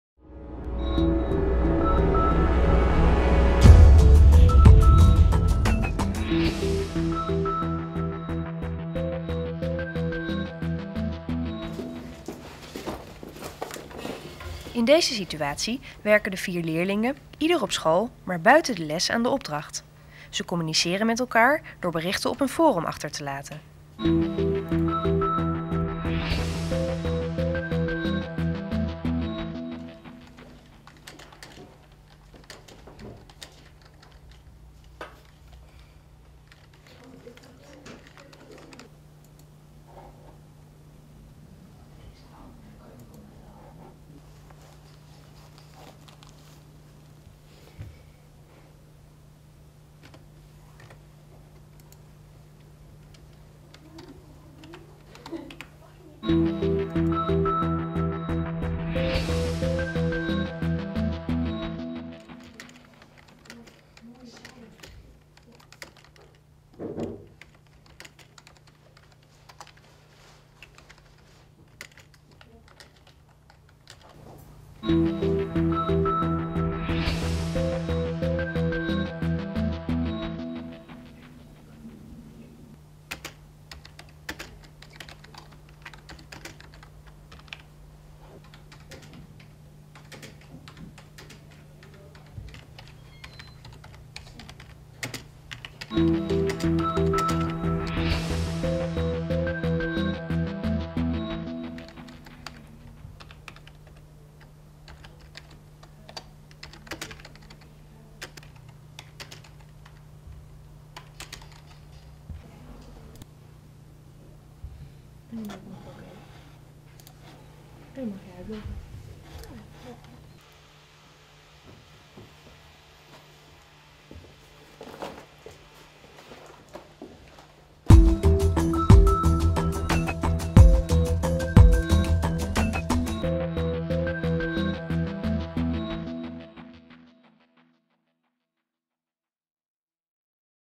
Computerondersteund samenwerkend leren - Praktijksituatie 4
In deze situatie werken de vier leerlingen ieder op school, maar buiten de les aan de opdracht. Ze communiceren met elkaar door op een forum berichten achter te laten.